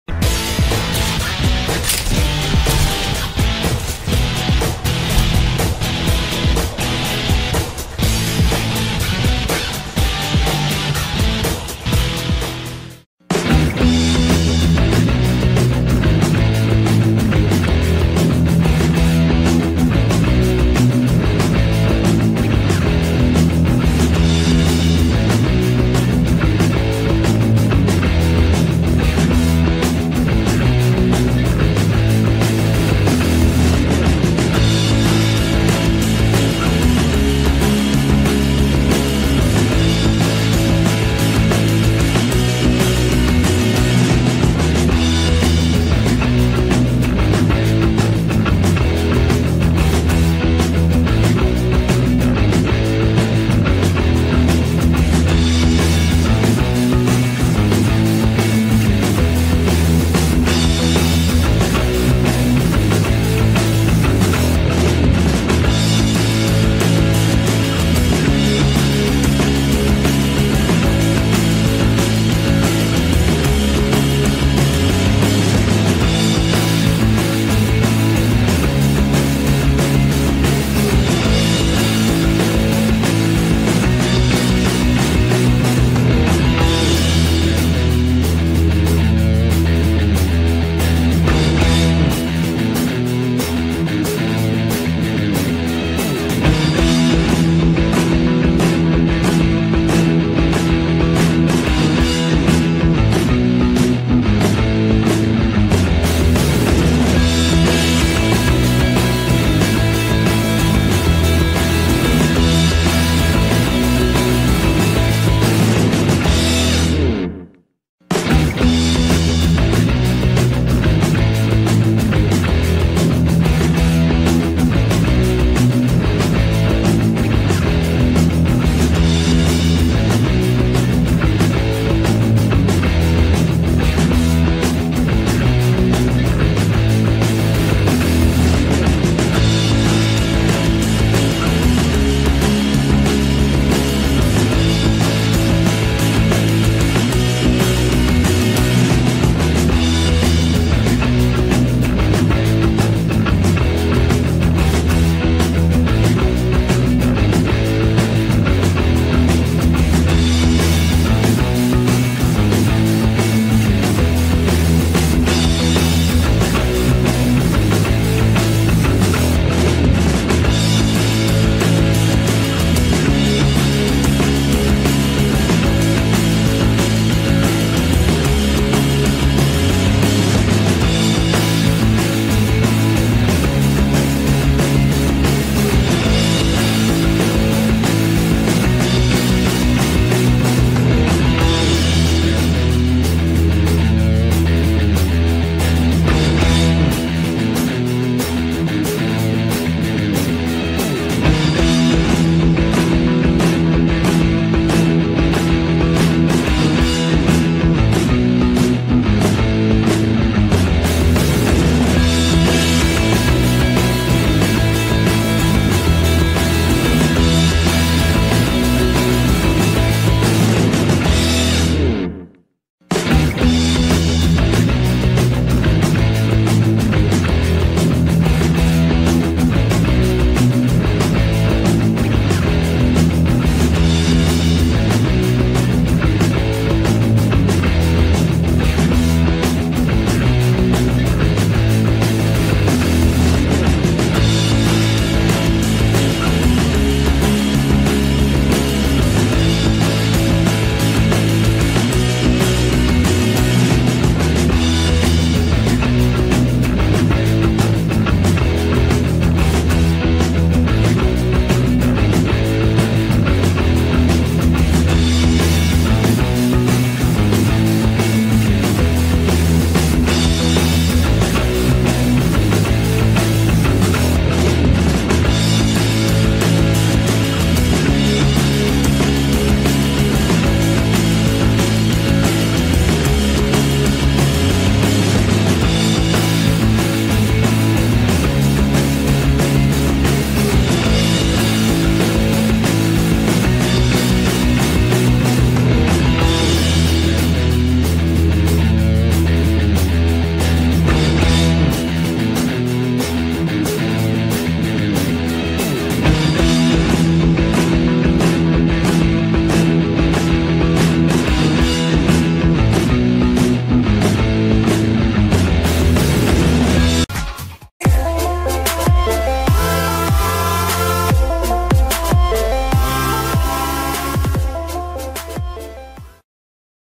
Katia Ricciarelli: “Pippo Baudo mi chiese di abortire. Poi fui punita, non ho avuto figli”
► Katia Ricciarelli si racconta, senza escludere particolari molto intimi, nell'ultima puntata del pri...